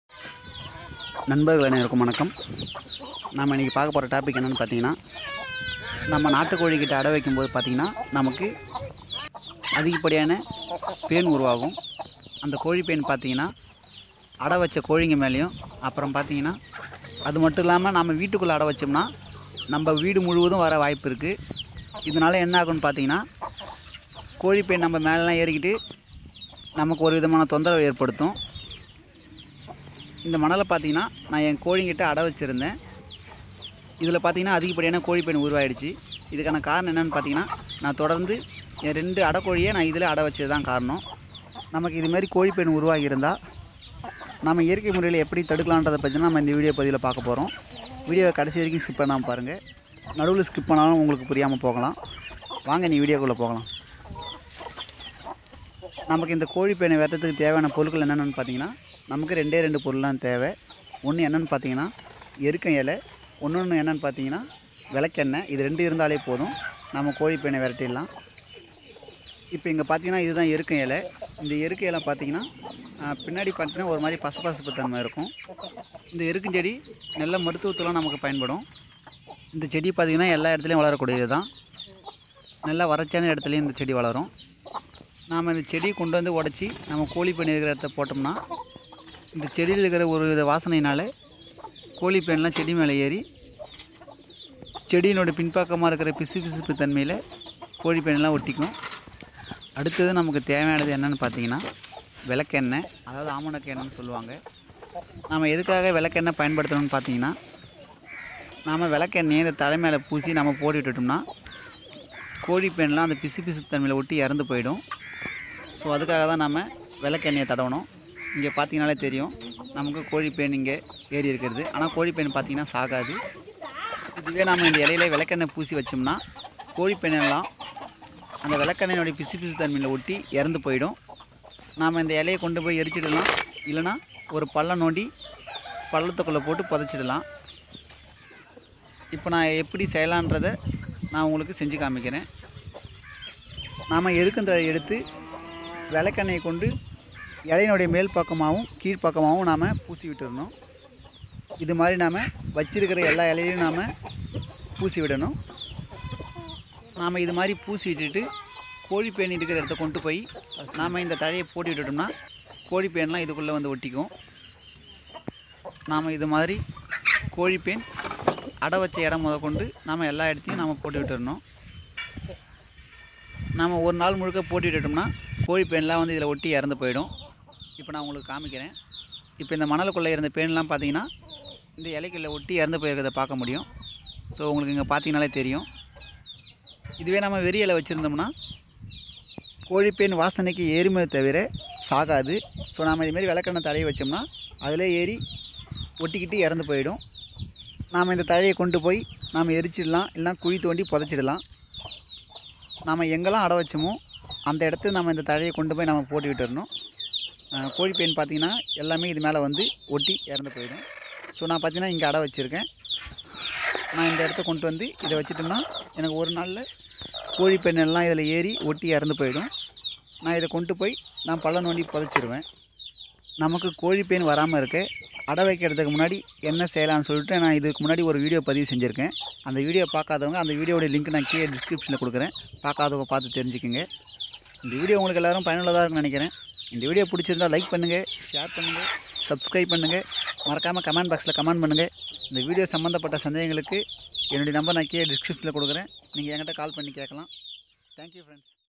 கோழி பேன் வந்தால் என்ன செய்யலாம்/how to stop hen lice in nature way.
இந்த வீடியோ பதிவு நாட்டுக்கோழிகளுக்கு ஏற்படும் பேன் தொல்லையிலிருந்து இயற்கை முறையில் எப்படி சரிசெய்வது என்பது பதிவு செய்யப்பட்டுள்ளது.